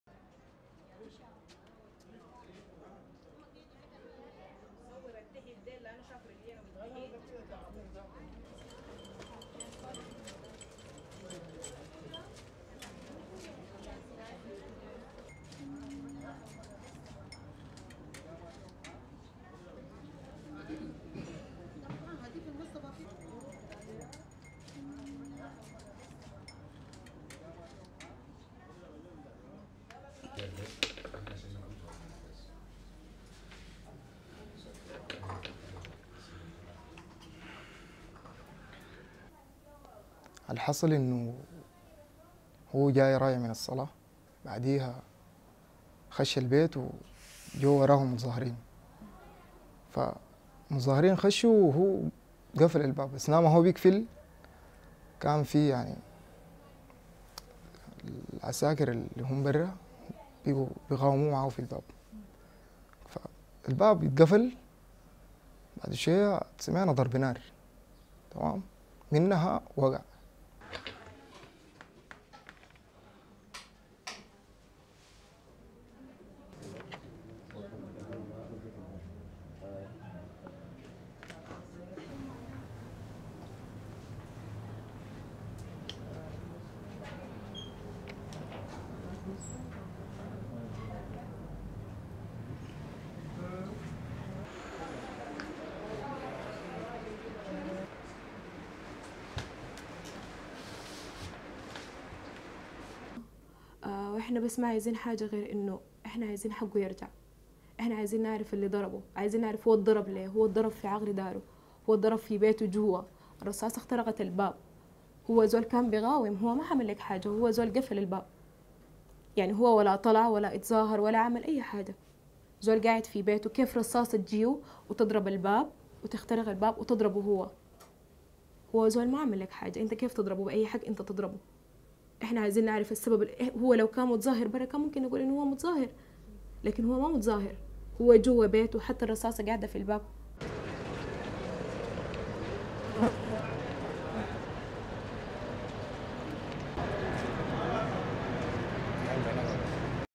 Sudan family says man shot sheltering protesters
(24 Jan 2019) The family of a Sudanese man who died amid a wave of unrest in the country say he was shot in his home as he tried to shelter protesters being chased by government security forces.
Maawiya Bashir returned home from prayers at a nearby mosque on 18 January, when several protesters ran through the front gate behind him, asking for shelter, according to his son Yahya.
He said his father agreed, and shut the door behind them, then tried to prevent armed members of government security forces from entering.
At one point, a shot was fired through the gate, hitting Bashir and knocking him to the ground.
"And then he fell", his son said.
The family said he died of the wounds in the hospital several hours later.
The protesters managed to escape over a wall.
Bashir is one of the estimated dozens of people who have been killed since the protests began over a month ago.
The protests began mid-December over price hikes but have morphed into calls for the President Omar al-Bashir, who has been in power for 29 years, to step down.
Since then, authorities in Sudan have used tear gas, rubber bullets, live ammunition and batons to quell the unrest.
They have imposed emergency laws and night-time curfews in some cities and suspended classes in schools and universities in others.
They have arrested opposition leaders, doctors, journalists, lawyers and students along with some 800 protesters.
Rights groups say at least 40 people, including children, have been killed in the clashes, most by gunshot wounds.
Al-Bashir's government has acknowledged only 24 deaths.